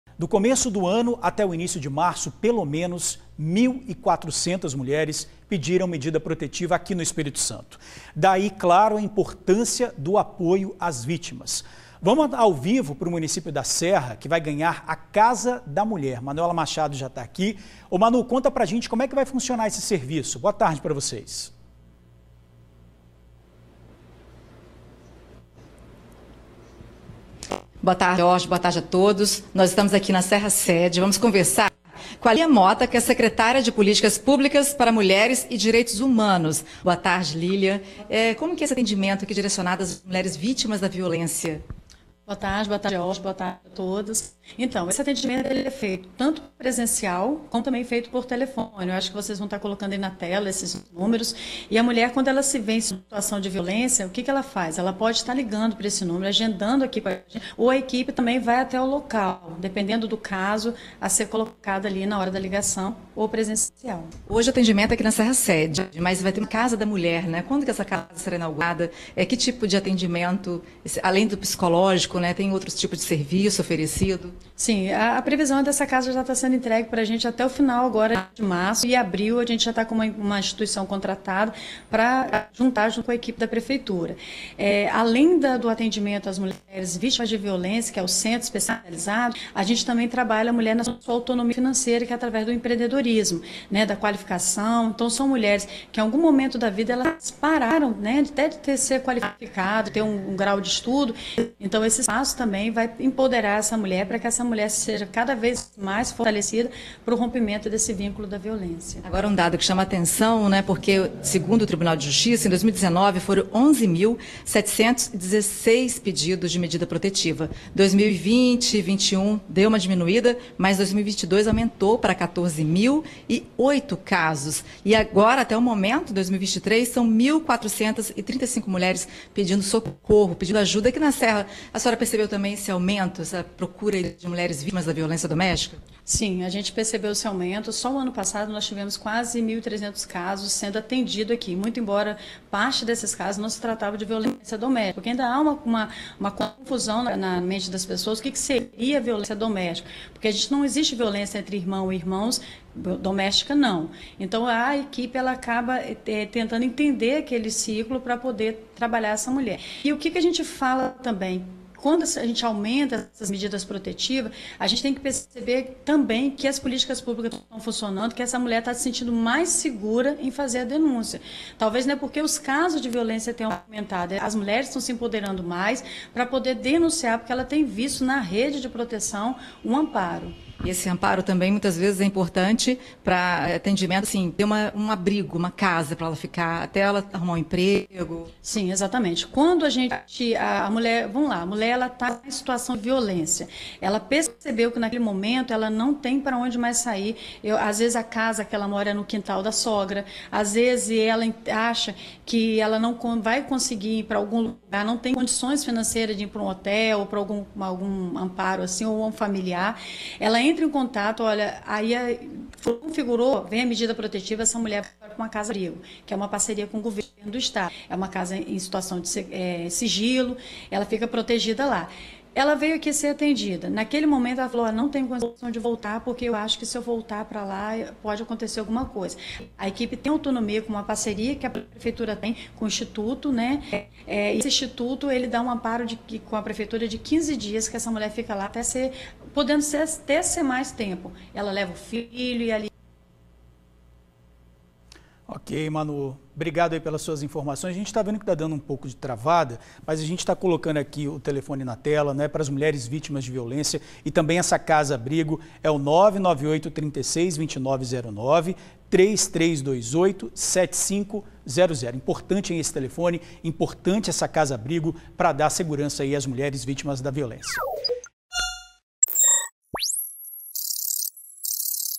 1.400 mulheres pediram medidas protetivas no ES em 2023
O município da Serra, na Grande Vitória, vai oferecer cursos que podem gerar renda às vítimas da violência.